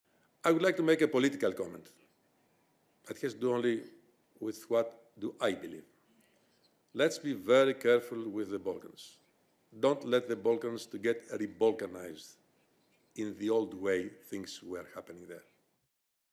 2019.10.17. - Albánia és Észak-Macedónia egyre messzebb kerül az EU-csatlakozástól
Franciaország és Hollandia is megvétózta a csatlakozási tárgyalások megnyitását az Európai Bizottság keddi ülésén.